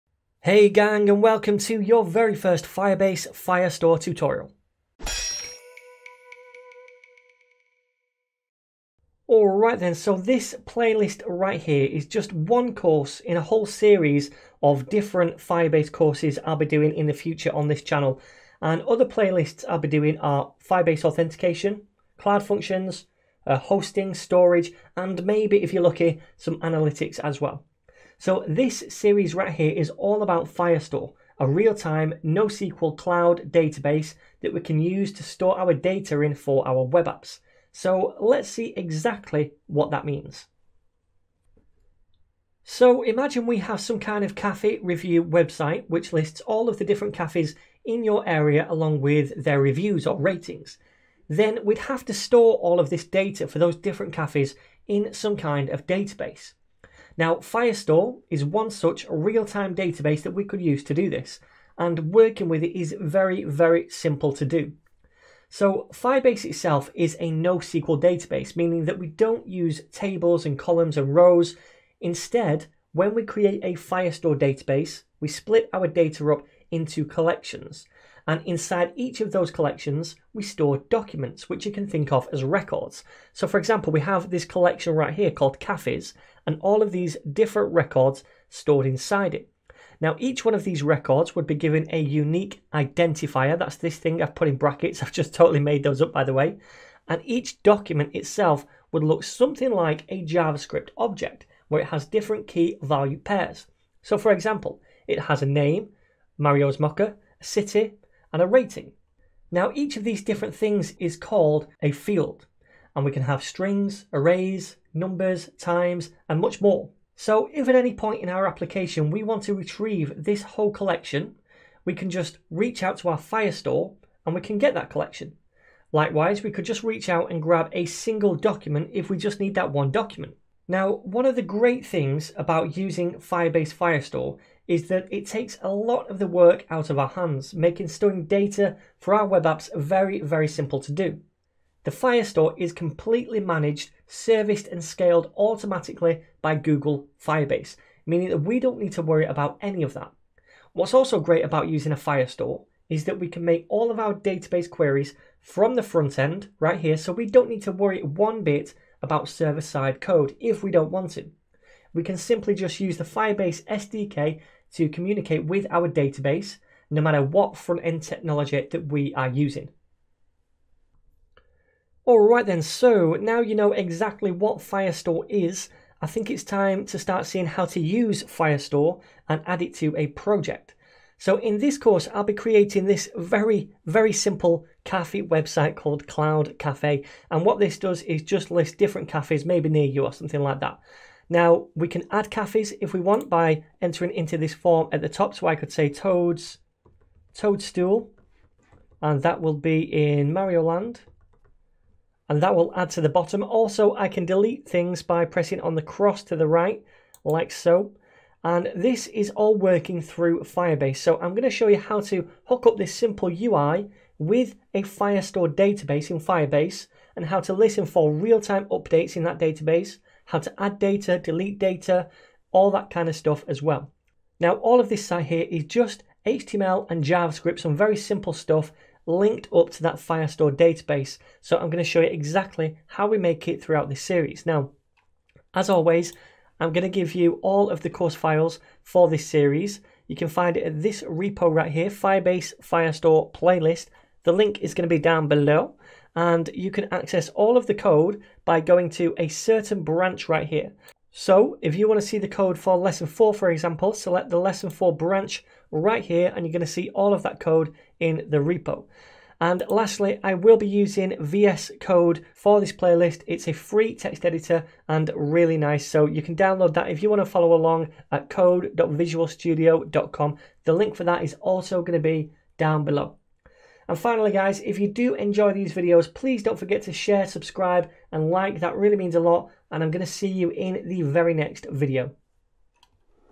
Firebase Firestore Tutorial #1 - Introduction
Hey gang, in this firebase firestore tutorial, I'll explain exactly what firestore is & why we'd want to use it!